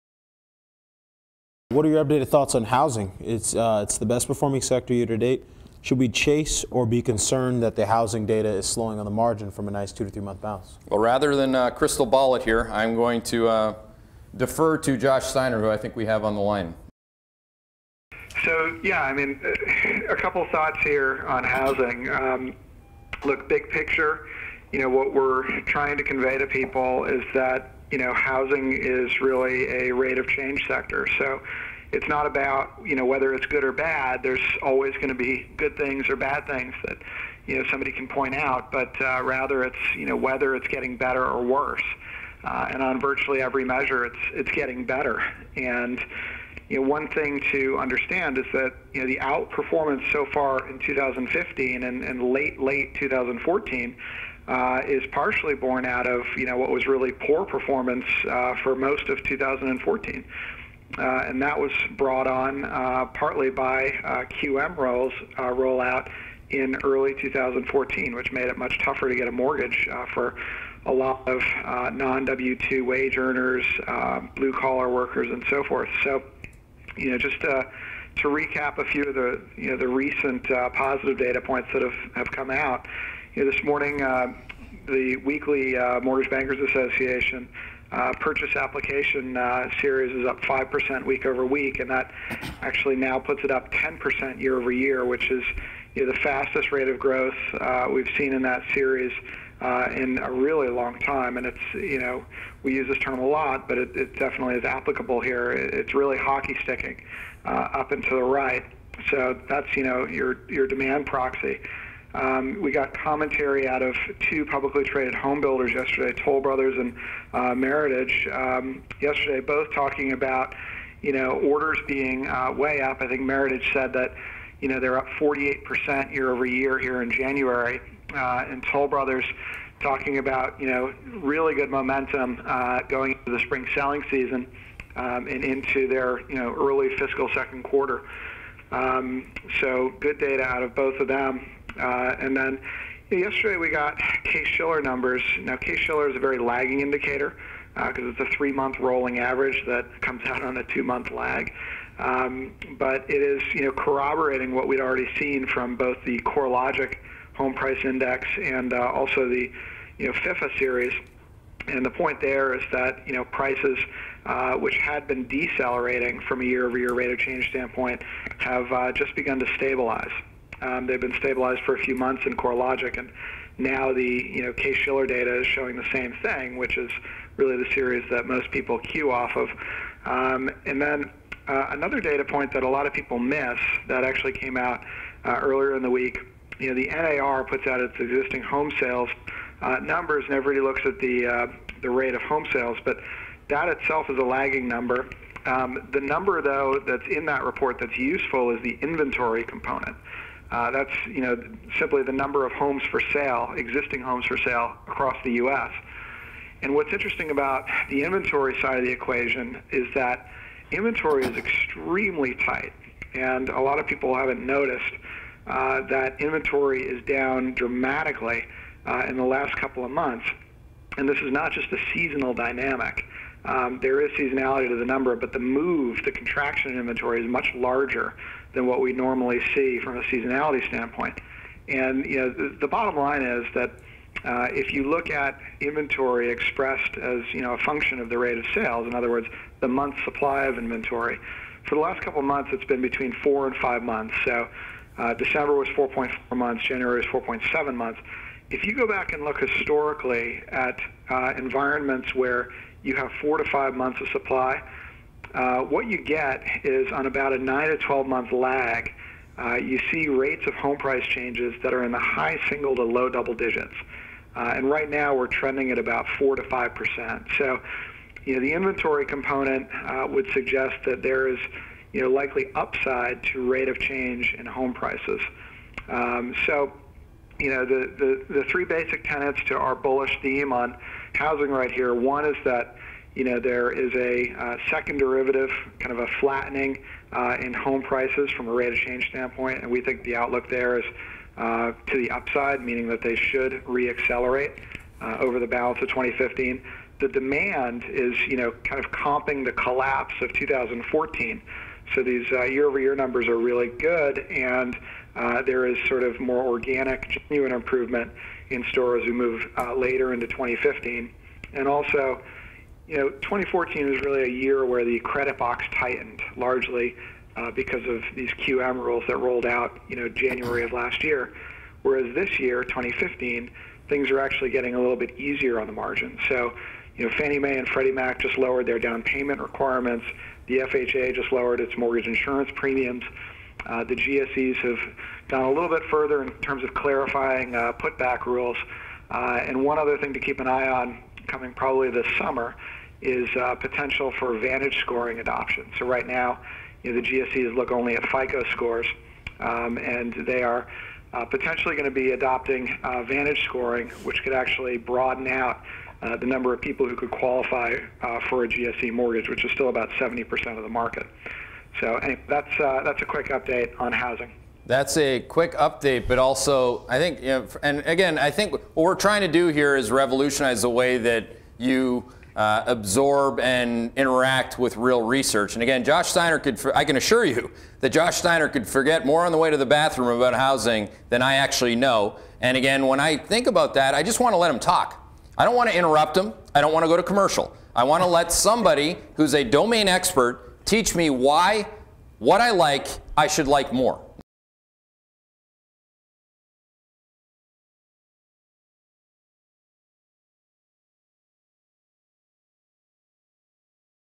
Housing Is The Best Performing Sector YTD…And We Still Like It
In this excerpt from this Morning’s Institutional Macro Call, Housing Co-Sector Head Josh Steiner explains in granular detail the primary reasons why we are bullish on housing.